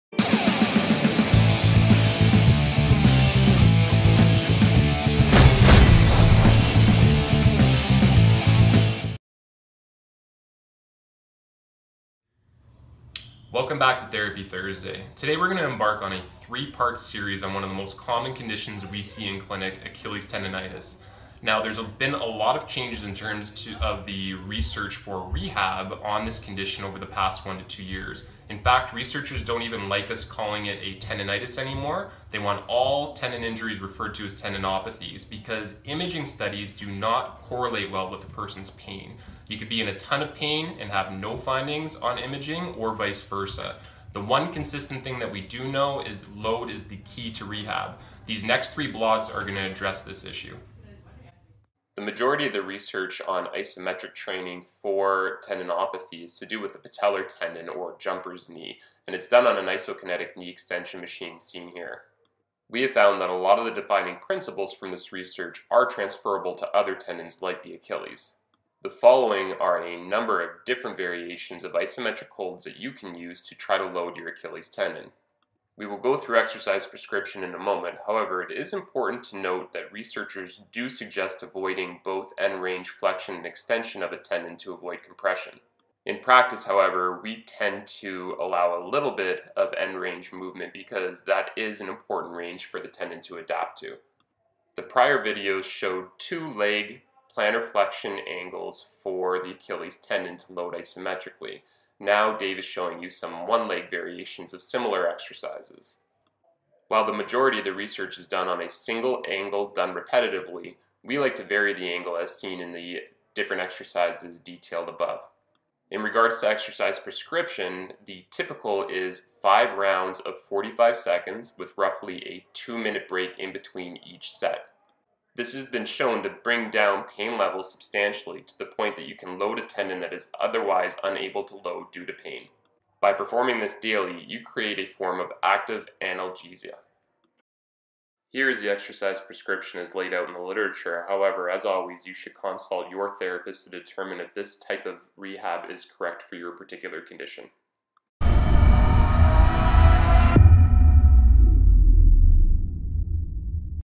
Achilles Tendonitis rehab part 1
This video blog describes some newer findings regarding tendinopathy rehabilitation specific to the achilles tendon.  We detail a 'phase 1' approach to exercise rehab based on recent research.  Blogs 2 and 3 will follow subsequent phases.